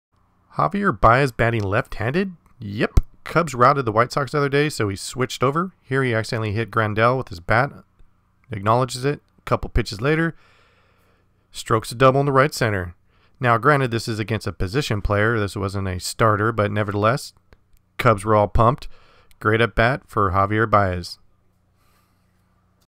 Javier Báez bats left handed during game vs WhiteSox
** Javier Báez and the Cubs took it the the White Sox on Sept. 25th. In the process of being blown out, WhiteSox put in a position player to save innings on their pitchers. So this gives Javier a chance to show off his talents by batting left handed. Granted the pitcher was throwing (50-60) mph, its still makes for great baseball.